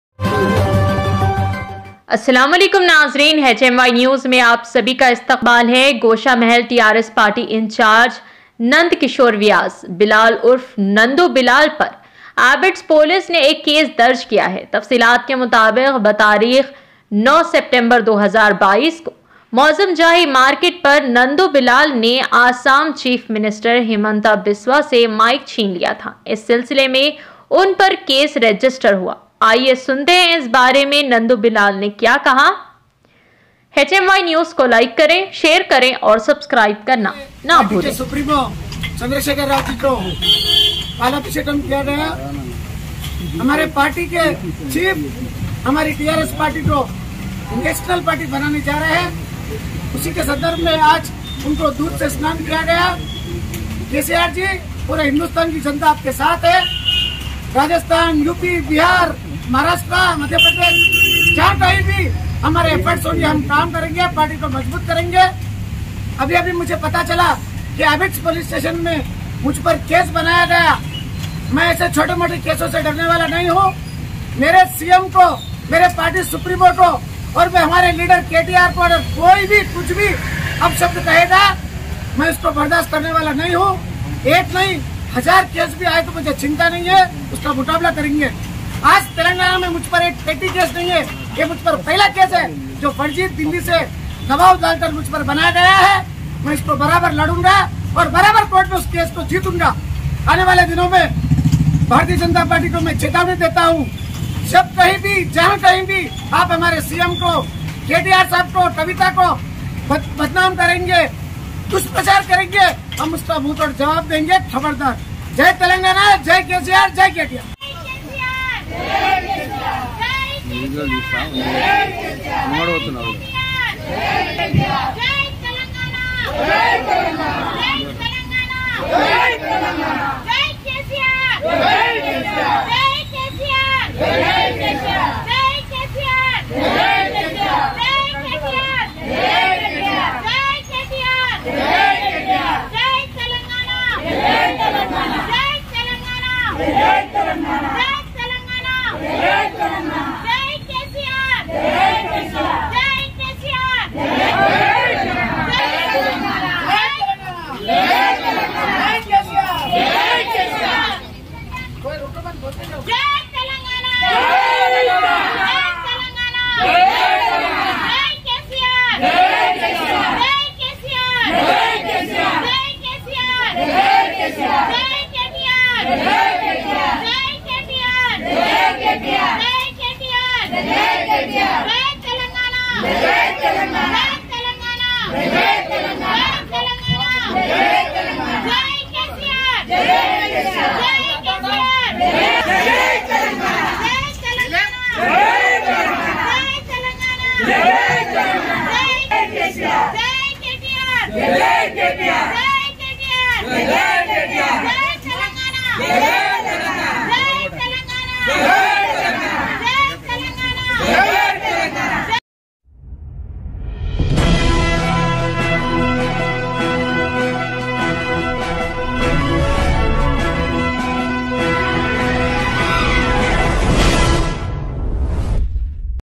Incharge Nand Kishore Vyas Bilal Urf Nandu Bilal Par Abids Police Ne Ek Case
hmynews Goshamahal TRS PARTY Incharge Nand Kishore Vyas Bilal Urf Nandu Bilal Par Abids Police Ne Ek Case Darj Kiya Hai

Tafseelat Ke Mutabiq 09/Sep/2022 Ko Mozam Jahi Market Par Nandu Bilal Ne ASSAM Cheif Minister Hemanta Biswa Se Mike Chin Liya Tha

Isi Silse Me Unpar Case Register Hua

Aaye Sunte Hai Is Bare Me Nandu Bilal Ne Kya Kaha